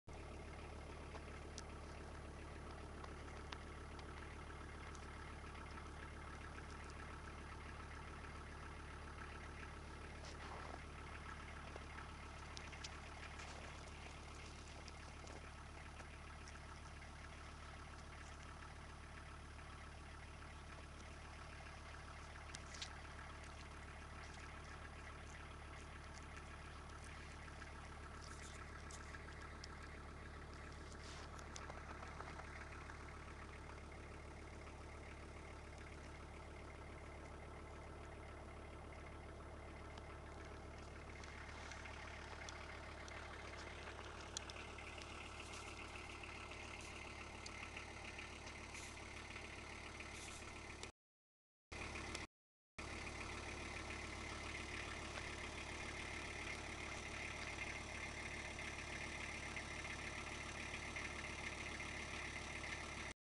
Otter in Tromsø
An Otter eats a dead frozen reindear, in Kattfjord, outside Tromsø, Arctic Norway.